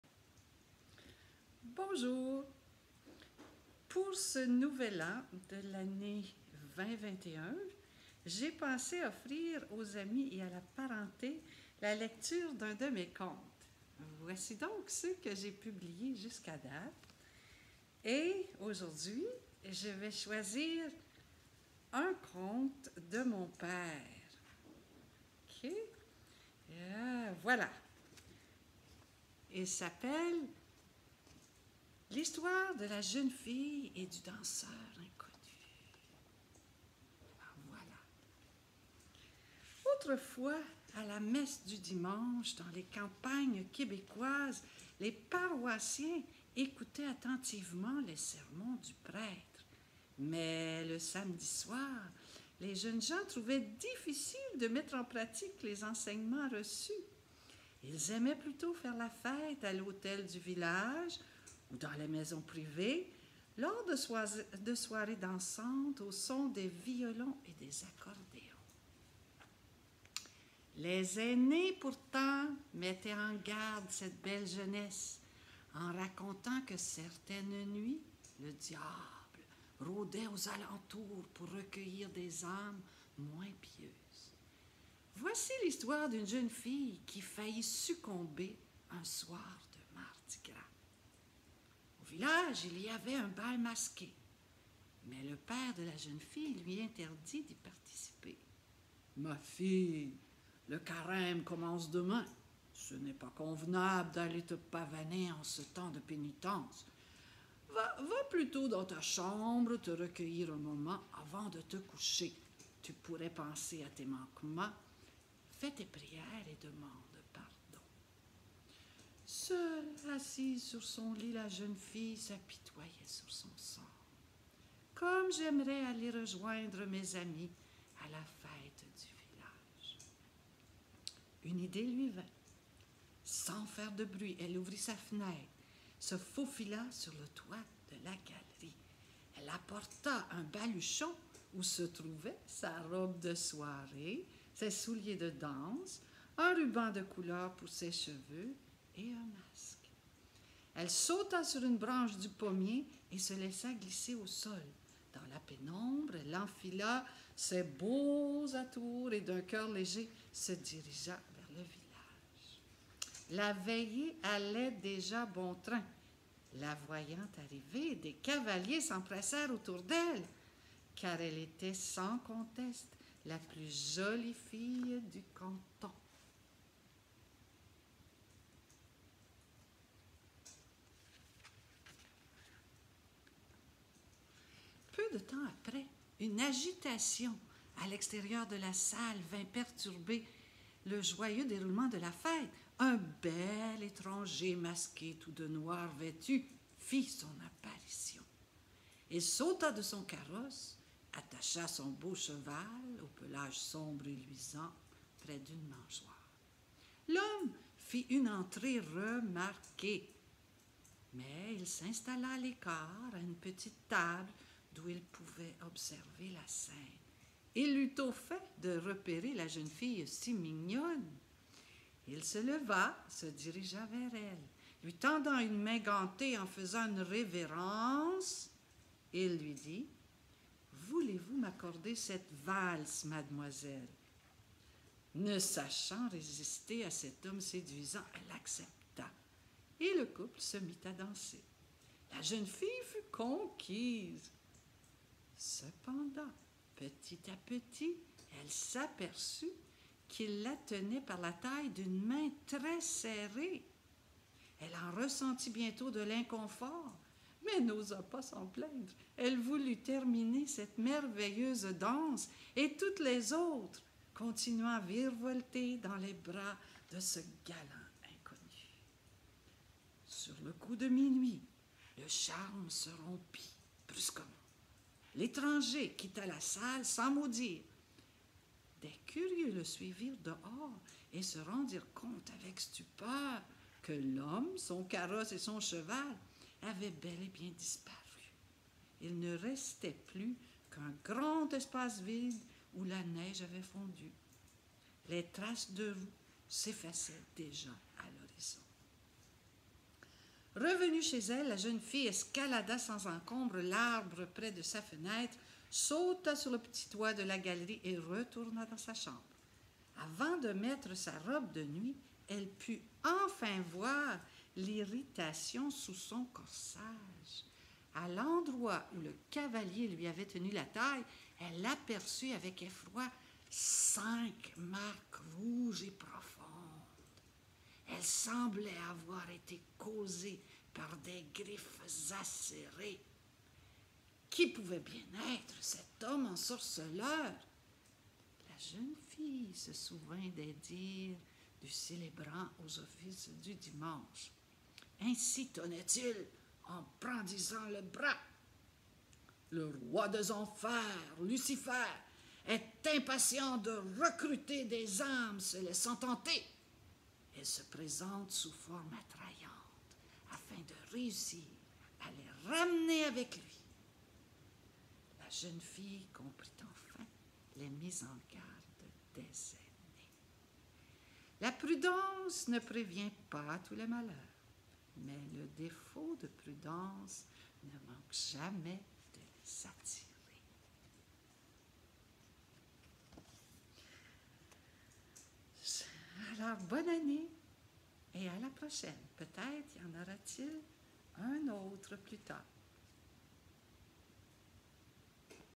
Les contes de mon père
Voici  un conte populaire québécois intitulé, L’histoire de la jeune fille et du danseur inconnu, écrit par Lorraine Hacquard, illustré par Diane Duclos et présenté par Lorraine Hacquard.